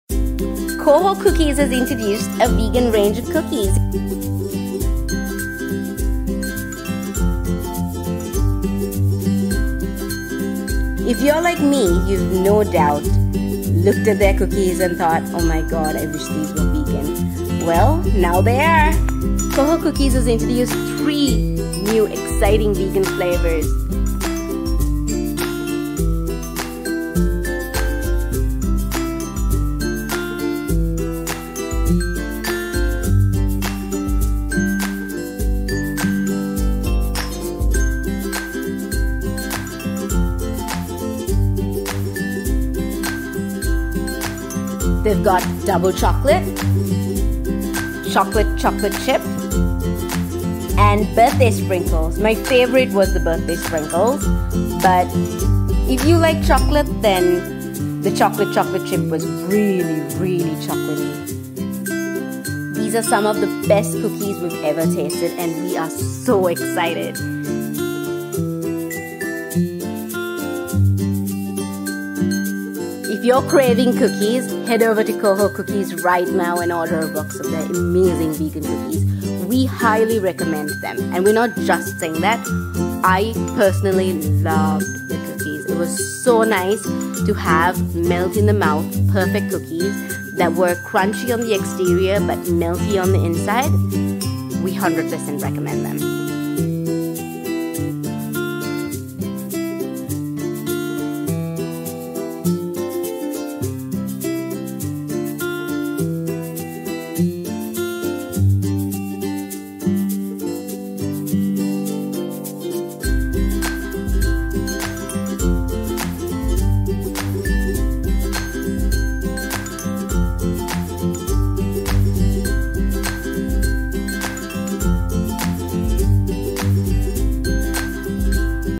Coho Cookies launches Vegan cookies!
Coho cookies has FINALLY launched a range of vegan cookies and we are loving them!